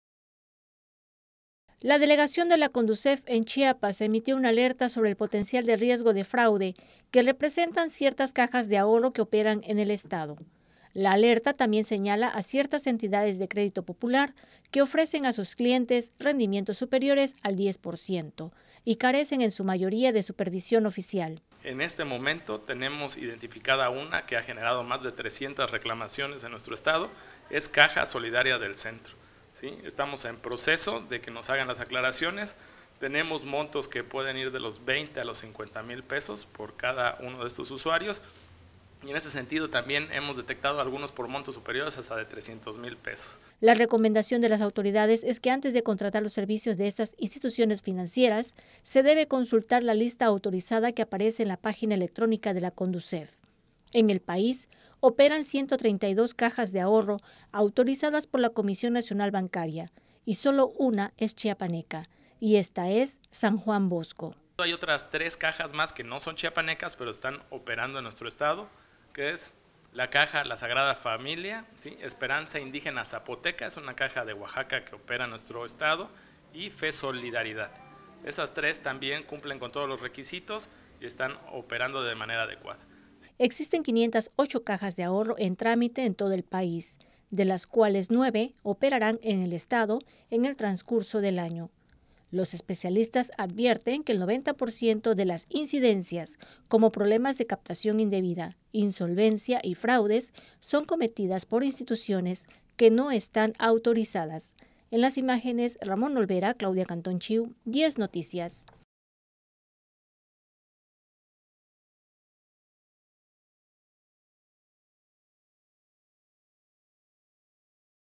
La Condusef en Chiapas emite alerta sobre potencial de riesgo de fraude en cajas de ahorro
La alerta también señala a ciertas entidades de crédito popular que ofrecen a sus clientes rendimientos superiores al diez por ciento y carecen en su mayoría, de supervisión oficial.
La recomendación de las autoridades es que antes de contratar los servicios de estas instituciones financieras se debe consultar la lista autorizada que aparece en la página electrónica de la Condusef.
En el país operan 132 cajas de ahorro autorizadas por la Comisión Nacional Bancaria y solo una es chiapaneca, la Caja Popular San Juan Bosco.